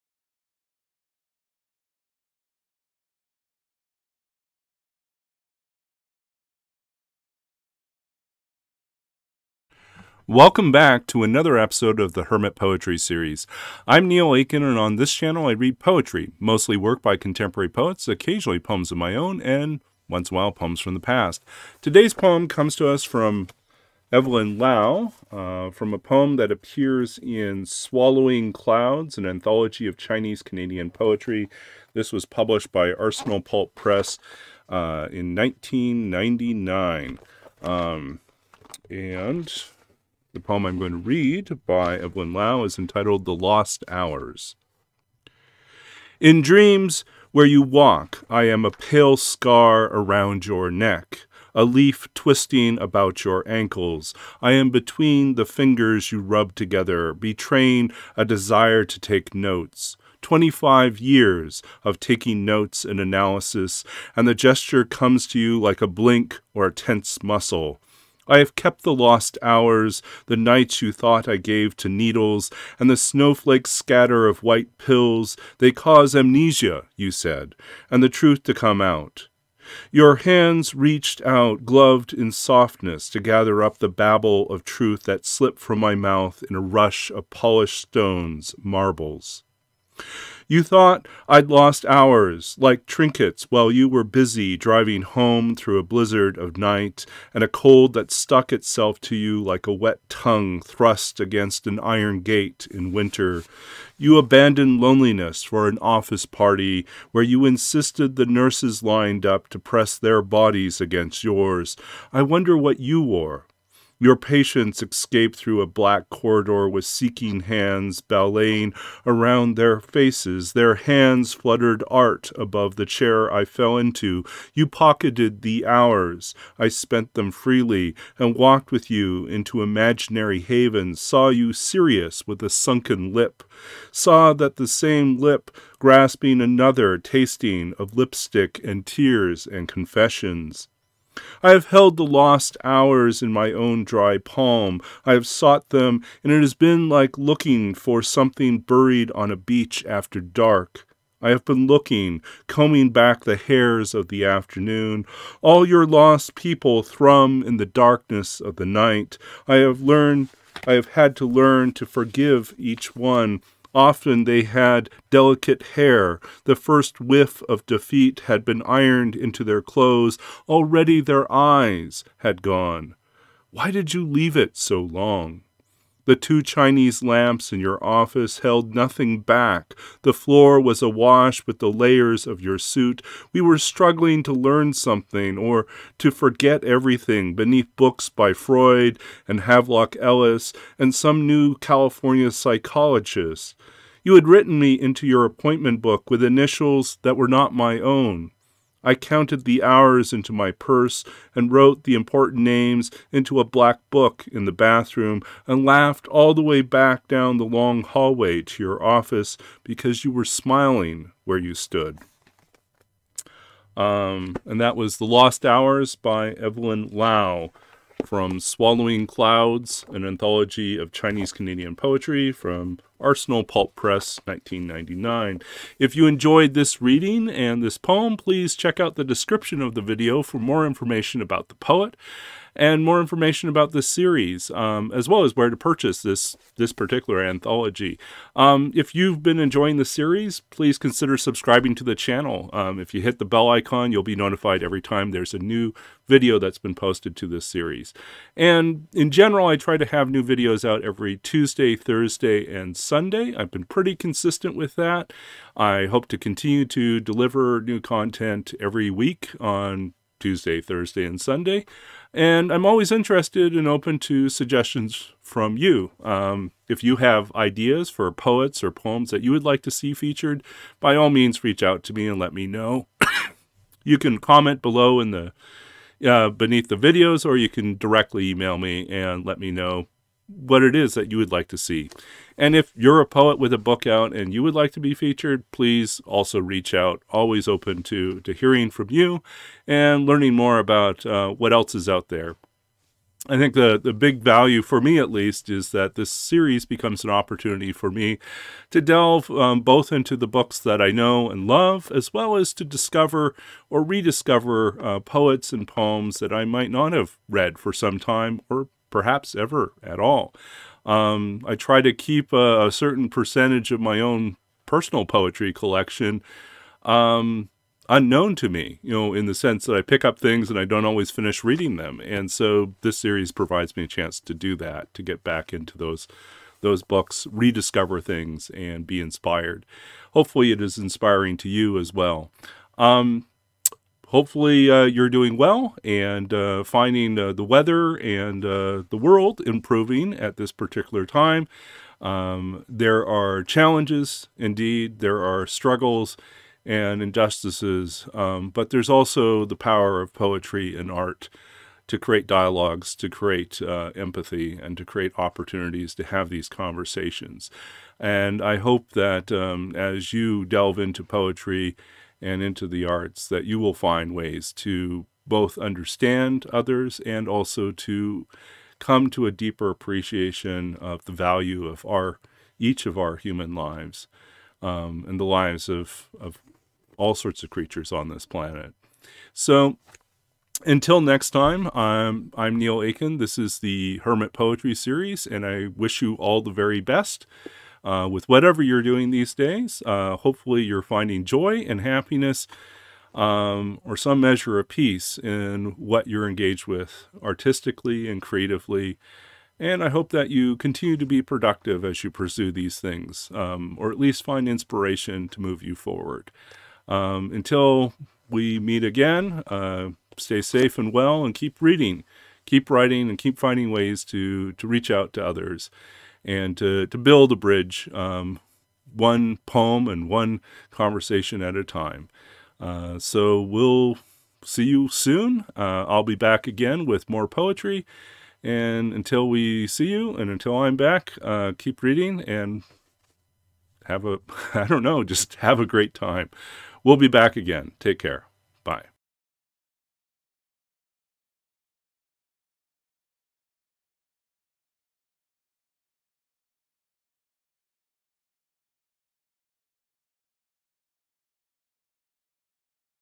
Hermit Poetry E182 - "The Lost Hours" by Evelyn Lau
Neil Aitken reads "The Lost Hours" by Evelyn Lau from the anthology Swallowing Clouds: An Anthology of Chinese-Canadian Poetry (Arsenal Pulp Press, 1999).

Evelyn Lau was born in Vancouver in 1971. She is the author of four volumes of poetry, two works of non-fiction, two short story collections and a novel. Runaway: Diary of a Street Kid, published when she was 18, was a Canadian best seller and was made into a CBC movie starring Sandra Oh in her first major role. Lau’s prose books have been translated into a dozen languages worldwide. You Are Not Who You Claim won the Milton Acorn People’s Poetry Award; Oedipal Dreams was nominated for the Governor-General’s Award. Her work has appeared in over a hundred literary magazines, garnering four Western Magazine Awards and a National Magazine Award. She has also won the Air Canada Award for Most Promising Writer and the Vantage Women of Originality Award. Her poems have been included in the Best American Poetry and Best Canadian Poetry series. She has read from and discussed her work at literary festivals and universities around the world; she presently freelances as a mentor to aspiring writers through UBC’s booming Ground and SFU’s Writing and Publishing Program.